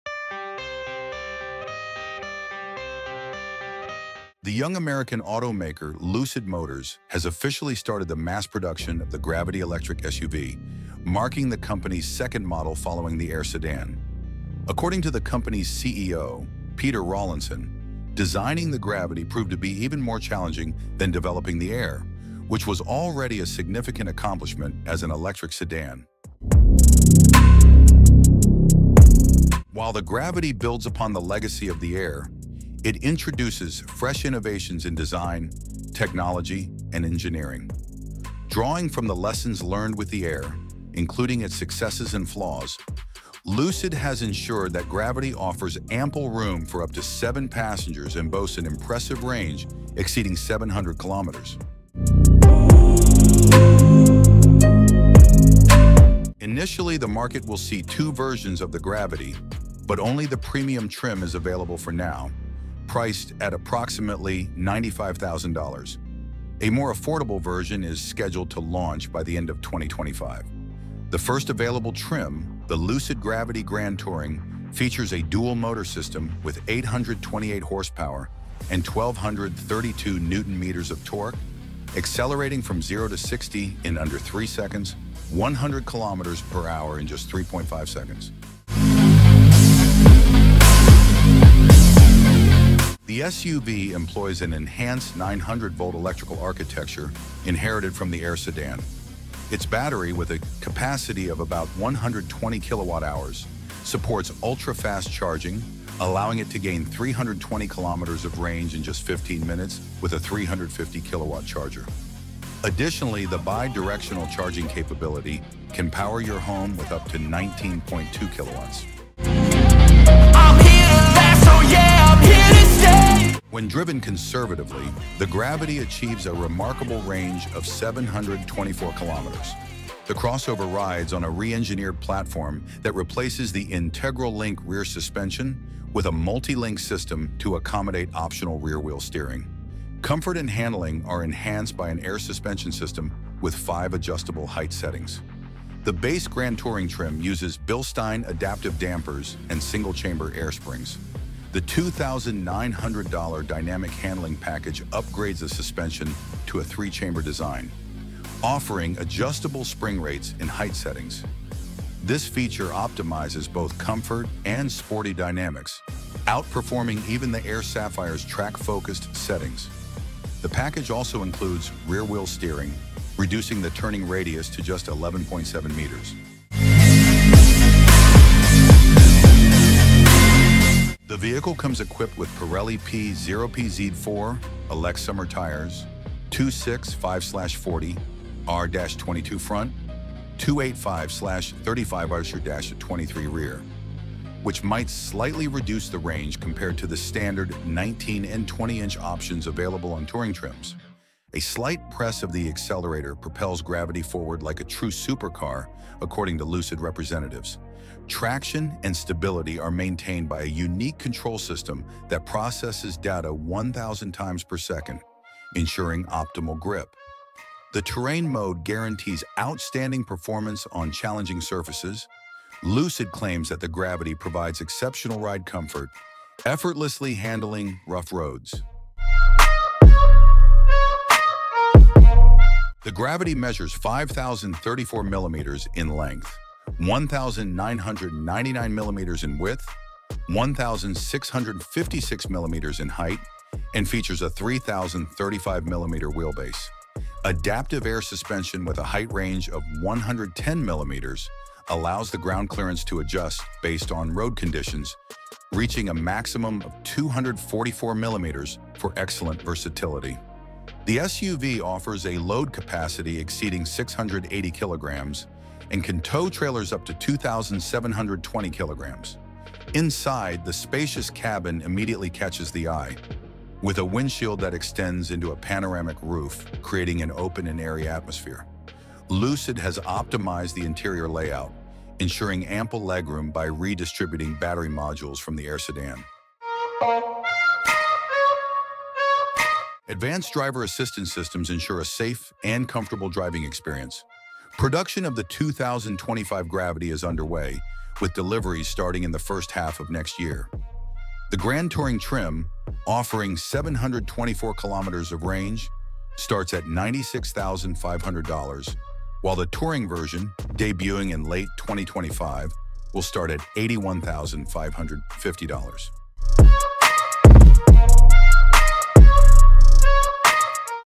2025 Lucid Gravity SUV: Electric Crossover of Dreams? The Future of Luxury Electric SUVs
2025 Lucid Gravity SUV: Electric Crossover of Dreams? The Future of Luxury Electric SUVs

The **Lucid Gravity 2025**, Lucid Motors' second model, sets a new benchmark in the world of electric vehicles. With spacious seating for up to 7 passengers and an impressive range of up to 724 km, this electric SUV combines comfort, power, and cutting-edge technology. The dual-motor **Grand Touring** version accelerates from 0 to 100 km/h in just 3.5 seconds, while its 350 kW fast charging system delivers 320 km of range in just 15 minutes. With an aerodynamic drag coefficient of 0.24, this large SUV boasts advanced features and a luxurious interior design. If you're eager to discover the future of automotive technology, the **Lucid Gravity 2025** is a must-see!